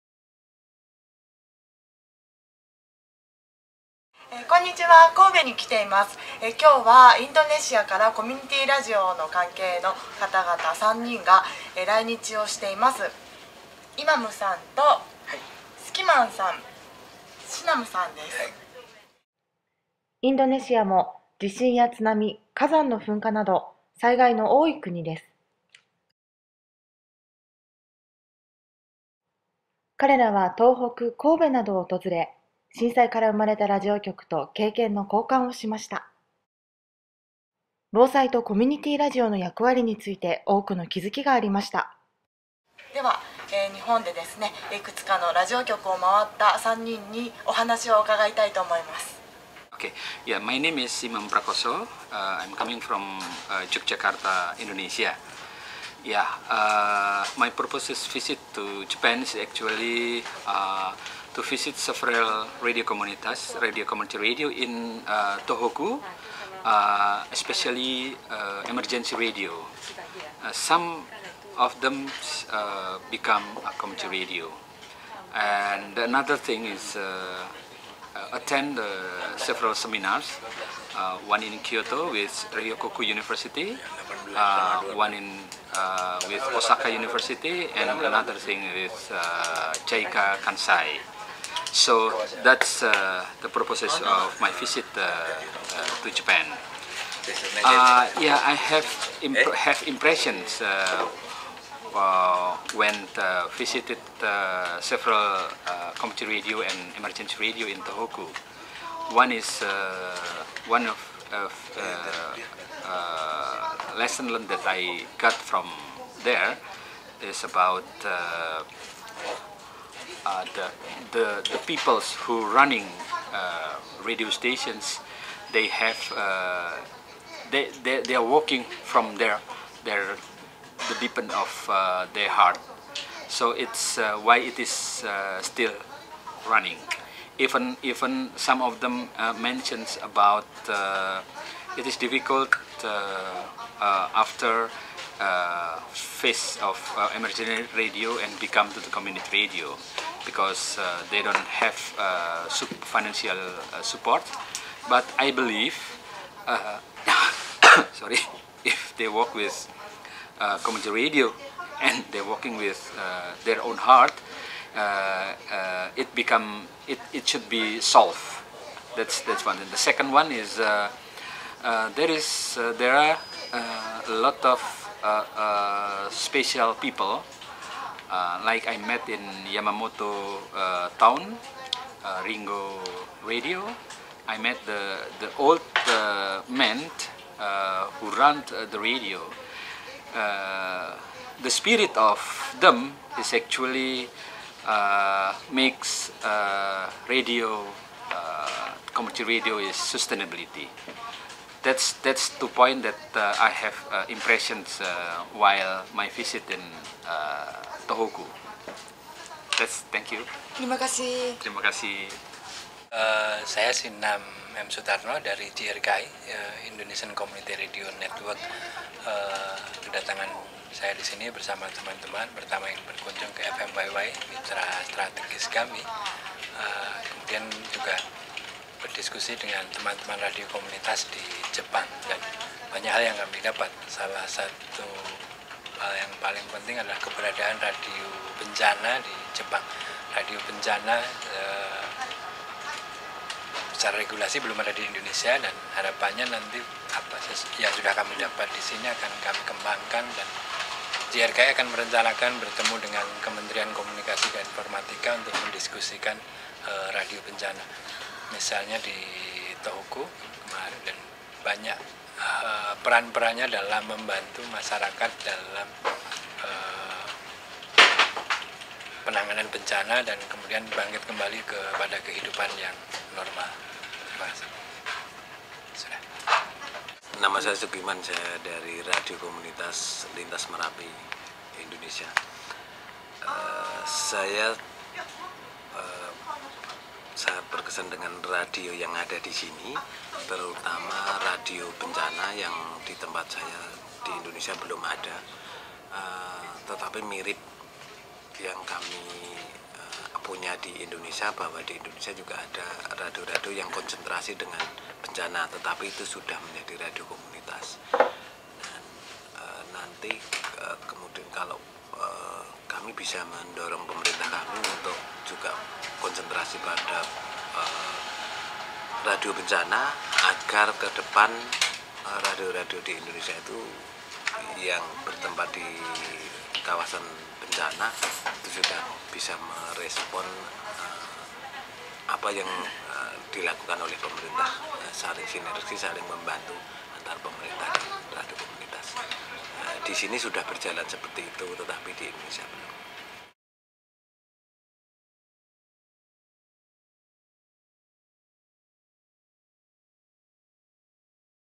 Community Radio ! (1)インドネシアから来日
【はっしーTV】インドネシアから来日したコミュニティFM関係者らにインタビュー。

インドネシアも地震や津波、火山の噴火など災害の多い国で約400局のコミュニティFMがあります。日本にも約270局あり、311以降の臨時災害FMの活躍などその存在価値に対する認識が広まっています。そして両国のコミュニティFM同士が恊働で地域の防災力UPのための取り組みが行われています。

今回は世界コミュニティラジオ放送連盟（AMARC）アジア太平洋地域支部理事でNGOコンバイン理事のイマムさん、インドネシアコミュニティラジオ協会代表のシナムさん、ムラピ山麓にある８つのコミュニティFMネットワーク・ジャリメラピ代表でリンタス村FM代表のスキマンさんの３人が来日。東北や神戸など震災から生まれたラジオ局を訪問し経験の交換が行われました。彼らは日本のコミュニティFMや被災地を訪れて何を感じたのか、どんなことを自国に持ち帰りたいと感じたのか、お話を伺いました。

2014年1月31日、神戸FMわぃわぃにて撮影。撮影協力：日比野純一さん、制作：橋爪明日香。動画5分52秒。

※英語とインドネシア語を訳してくださる方を探しています。